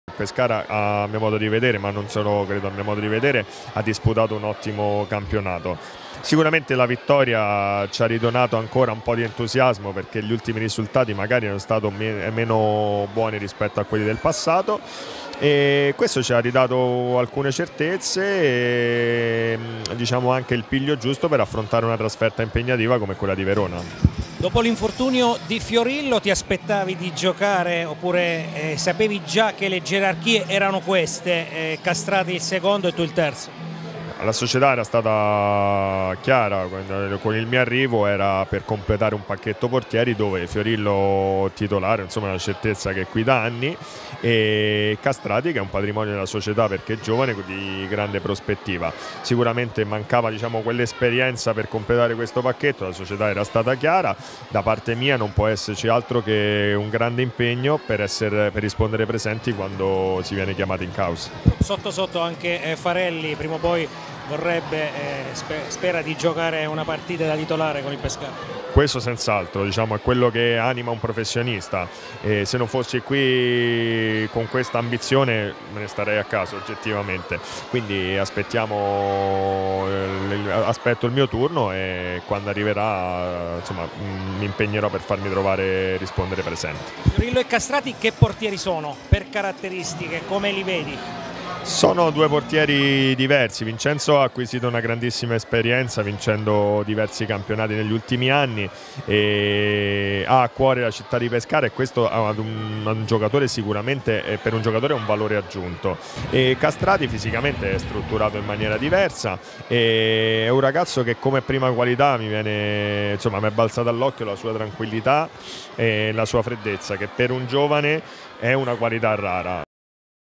Farelli: “La vittoria sul Carpi ci ha ridato certezze”
Seduta mattutina a porte chiuse al centro federale di Silvi per il Pescara che prepara il posticipo di lunedì sera del Bentegodi di Verona. La squadra partirà poi nel pomeriggio e sosterrà domattina la rifinitura allo stadio Mancini di Fano. Tra poco il tecnico Pillon, che non avrà certamente Fiorillo, Melegoni e Campagnaro, parlerà nella consueta conferenza pre partita. Intanto il portiere Simone Farelli parla della situazione dei biancazzurri e dell’importanza di aver ritrovato i tre punti nell’ultima giornata